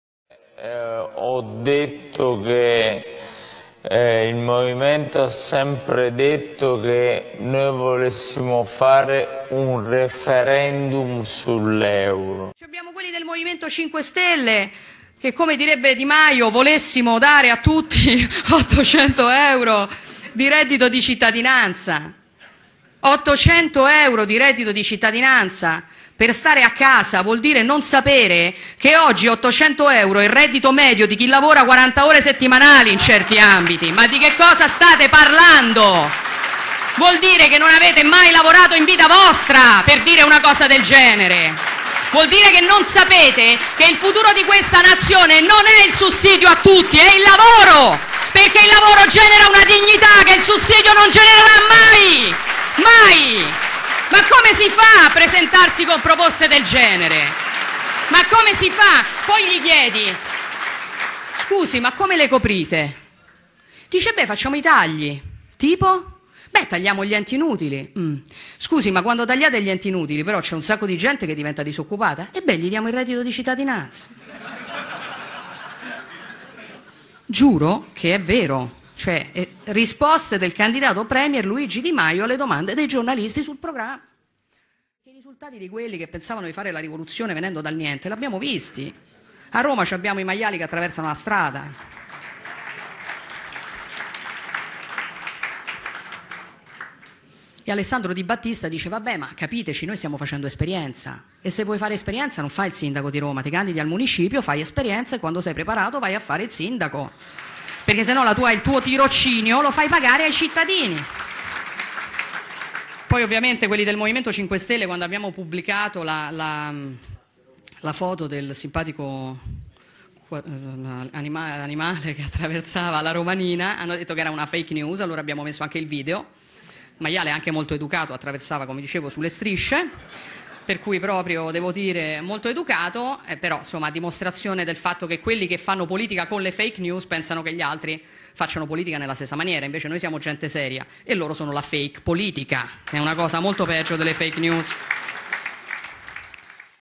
La Meloni sfotte Luigi "Mailavoratounora" Di Maio e gli smaschera il suo programma elettorale
Giorgia Meloni scatenattissima contro il reddito di fancazzismo 5 stelle